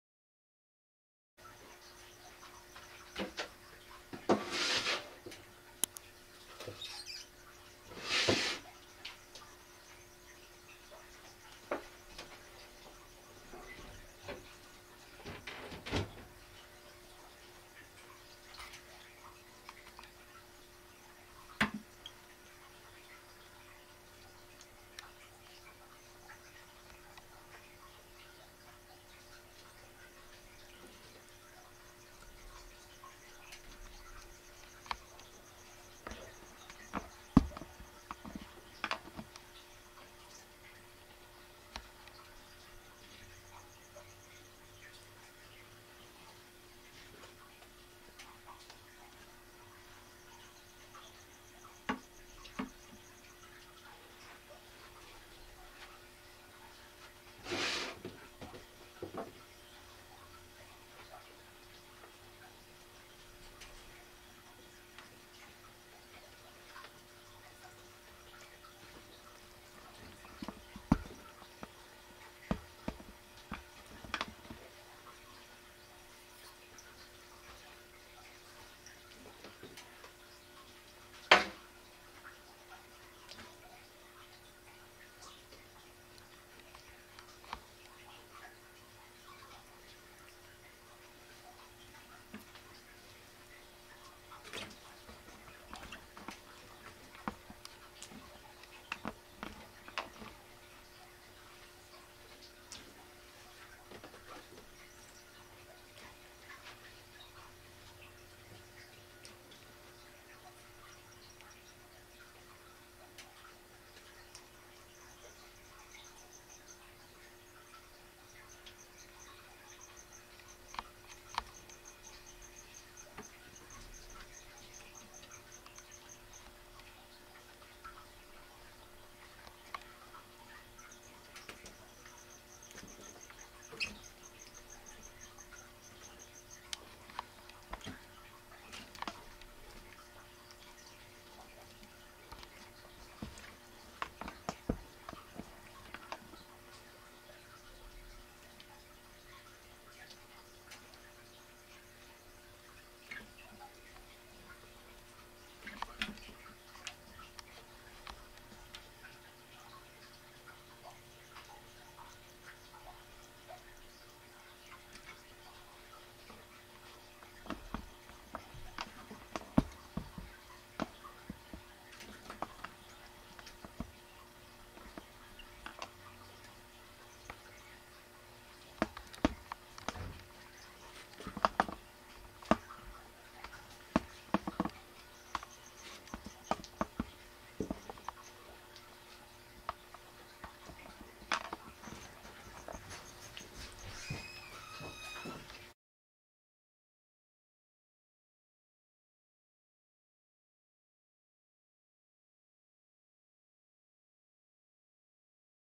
3 Baby Mice Eaten By Pixie Frog / Warning Live Feeding
in this video we document a pixie  frog eating a rat  .